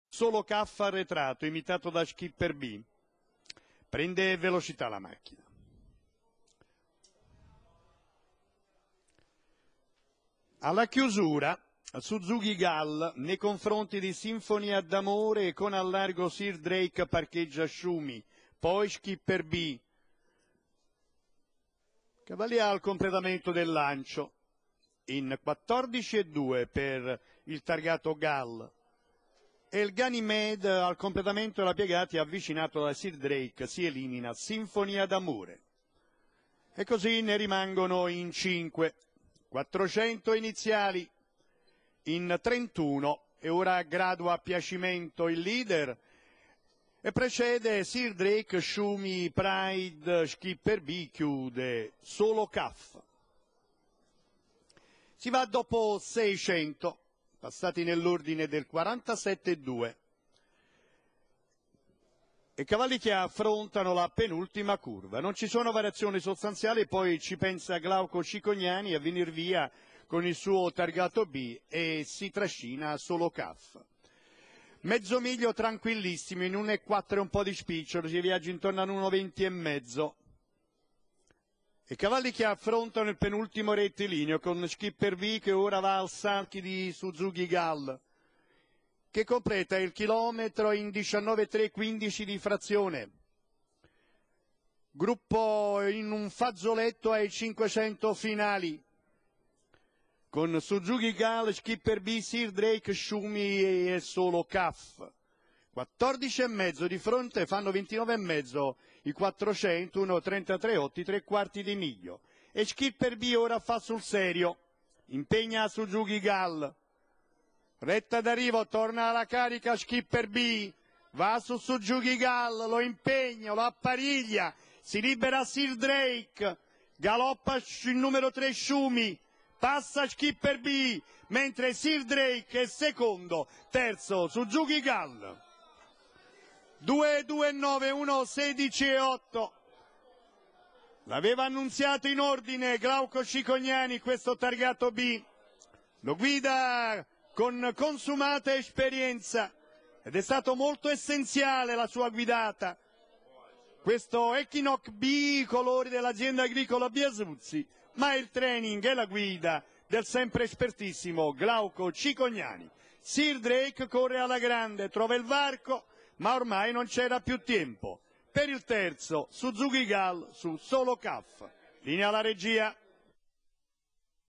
02/01/2014, Napoli , Premio Amalfi
1. SKIPPER BI (CICOGNANI GLAUCO), 2. SIRDREIK (D' ALESSANDRO GIORGIO SR), 3. SUZUKYGAL (DELL'ANNUNZIATA VP.), 4. SOLO CAF (DI STEFANO D.), 99. SINFONIA D'AMORE (ESPOSITO -O- ANT.), 99. SHUMY PRIDE (CASTALDO A.)